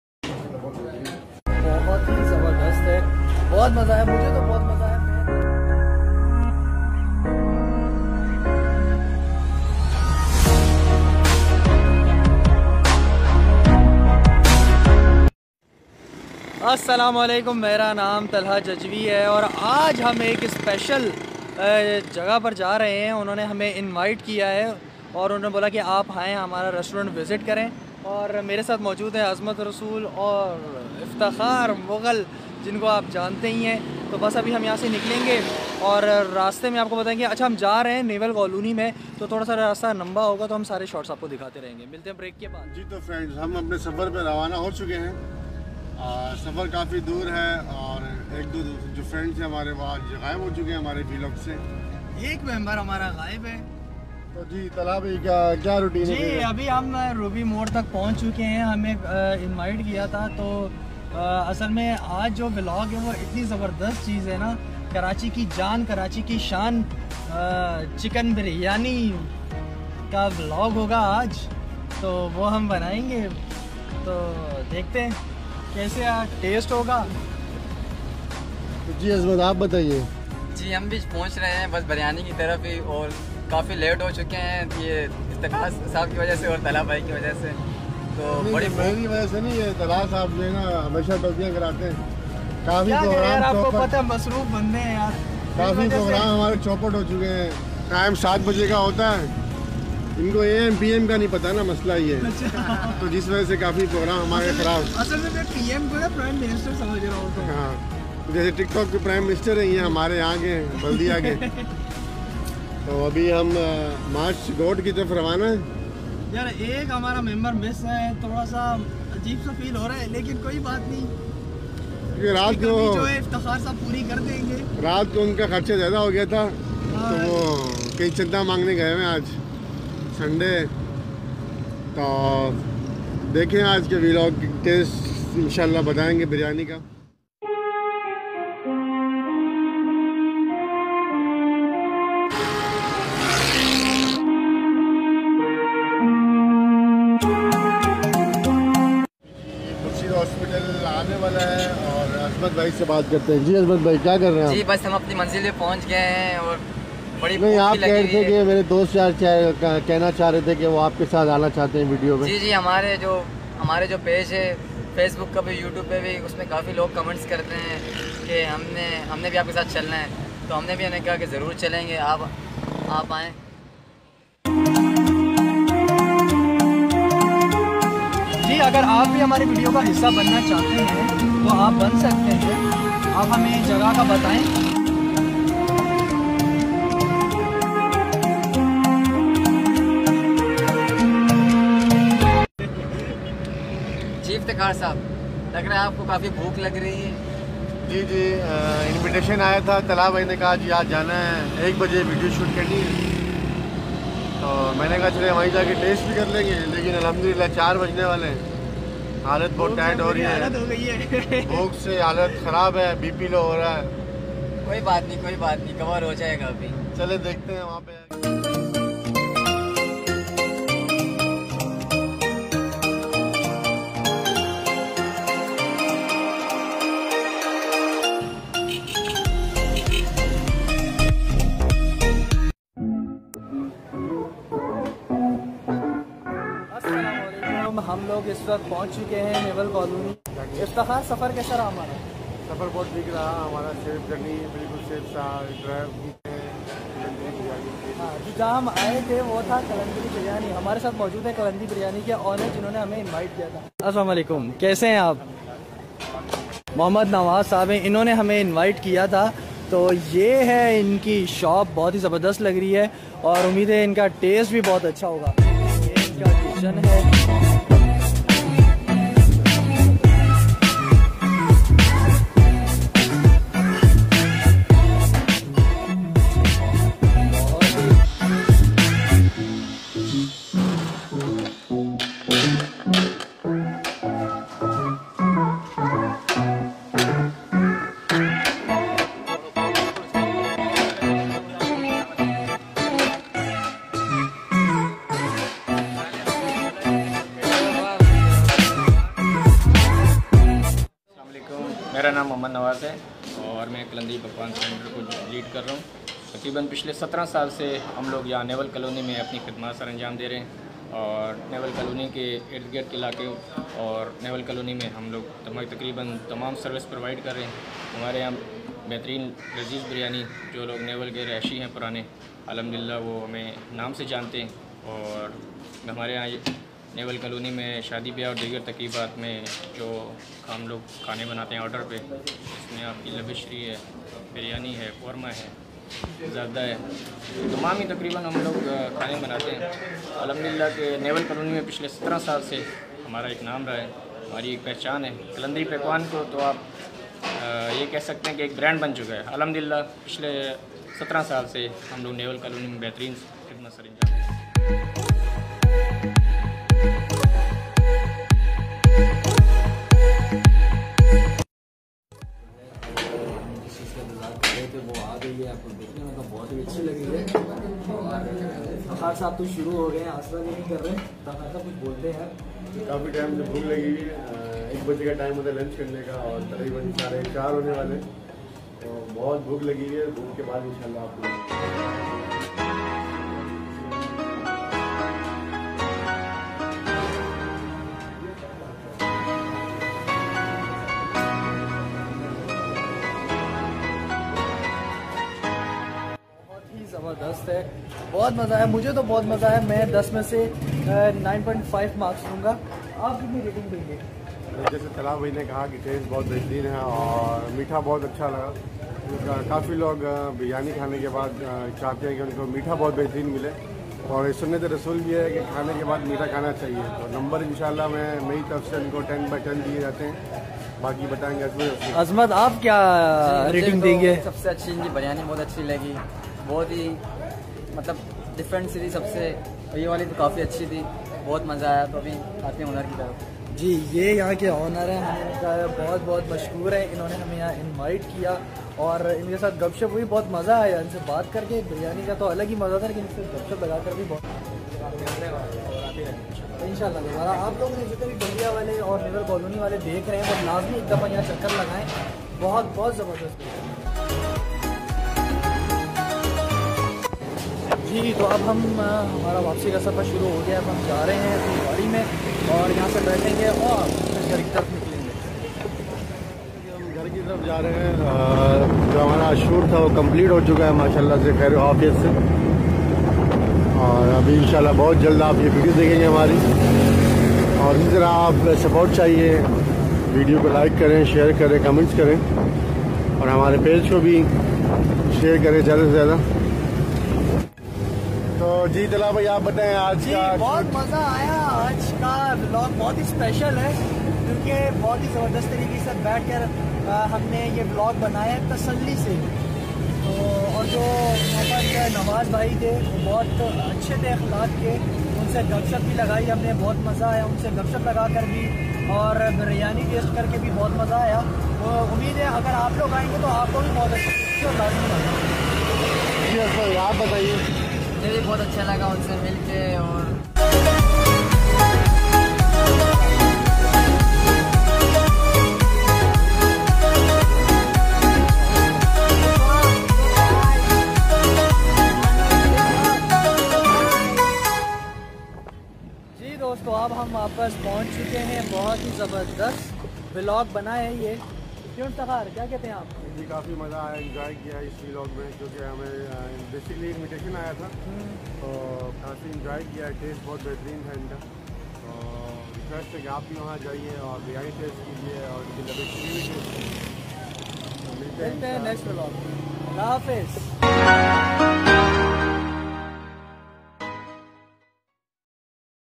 New Qalandari Biryani Karachi Baldiatown naval colony
New Qalandari Biryani

آپ تمام لوگو ں کی بھرپور فرمائش پر ٹیم نے نیول کالونی کی قلندری_بریانی کا ٹسٹ کیا ور ولوگ بھی بنایا امید سے زیادہ ہی اچھا بہترین ٹسٹ تھا بہت مزہ آیا محمّد نواز بھائی نے ہمیں دعوت دی تھی